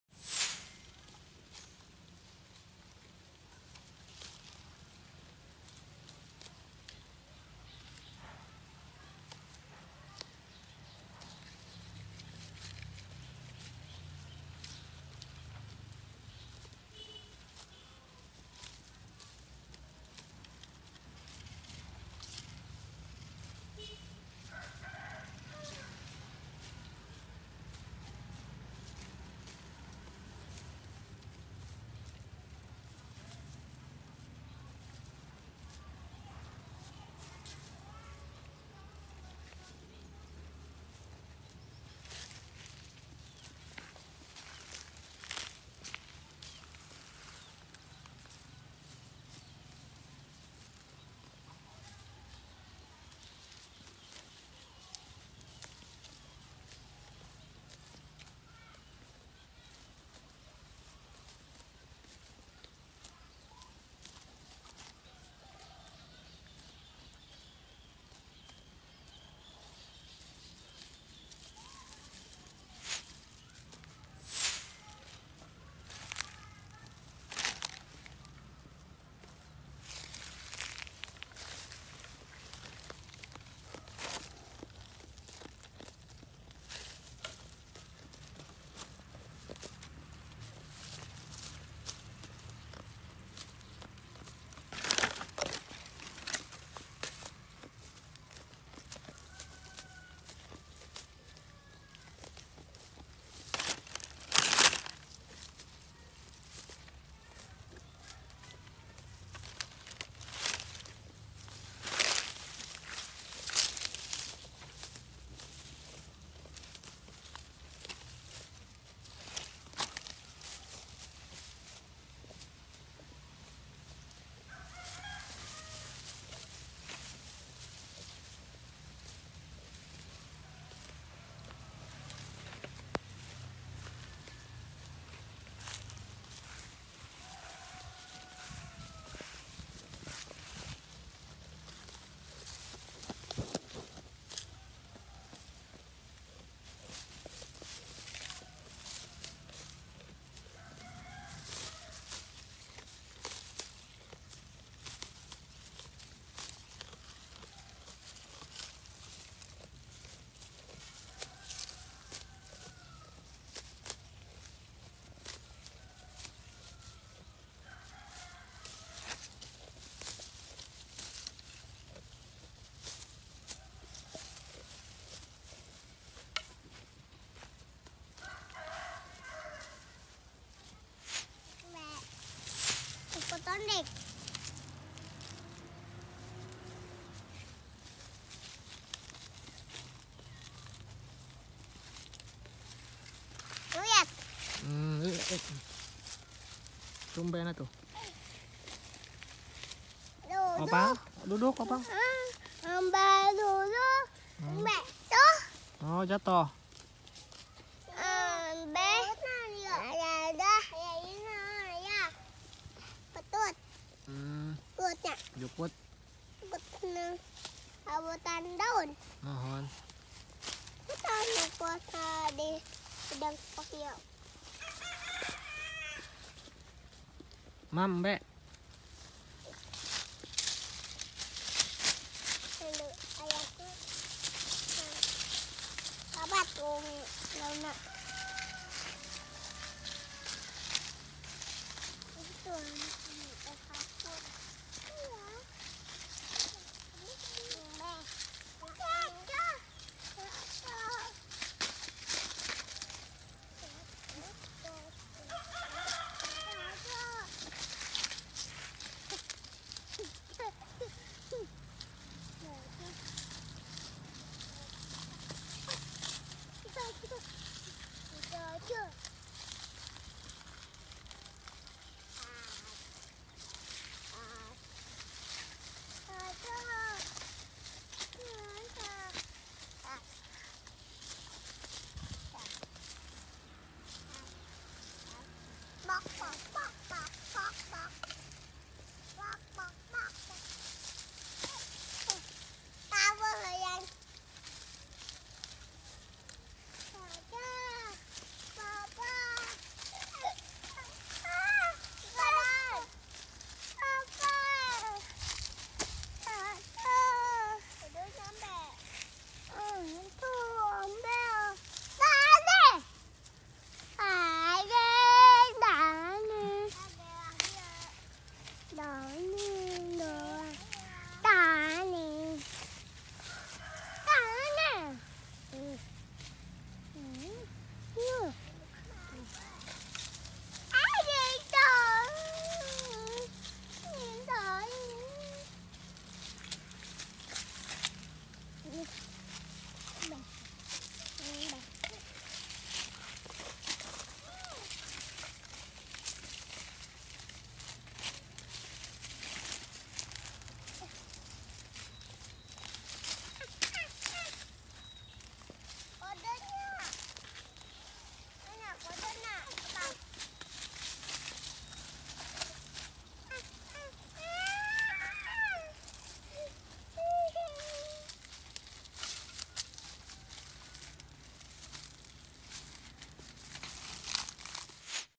Inilah Ternak Domba Tanpa Cape
Inilah Ternak Domba Tanpa Cape, tidak perlu ngarit 100% di angon, selain domba senang juga sehat.
syaratnya harus punya kebun rumput sendiri ya, kalo ga punya nanti bekeliaran ke kebun orang lain, mending kalo makan rumput aja coba kalo makan tanaman bisa bisa di marahin heheh.